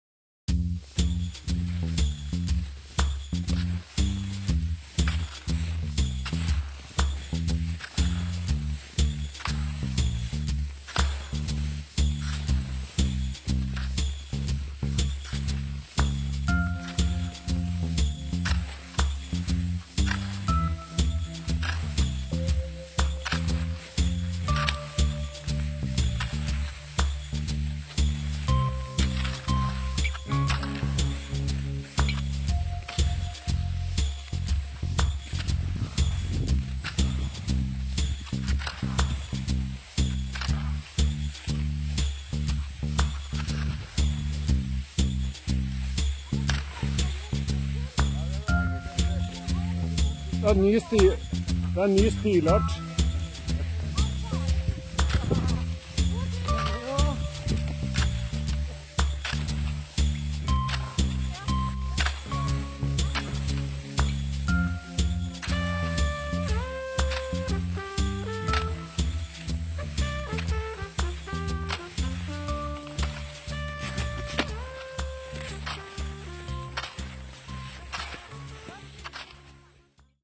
Bentes skitur med hunden blir sett av hundretusener: - Helt utrolig!
BESTEMT HUND: Skituren til Svein Erik Moe, Bente Forsberg og hunden Warro i Askim 3. mars, har fått mye mer oppmerksomhet enn ventet.
Etter at Svein Erik la ut denne snutten på en Facebook-gruppe for elgehundeiere, har responsen strømmet inn fra alle kanter. I skrivende stund har over 104 000 mennesker delt videoen. Video: Svein Erik Moe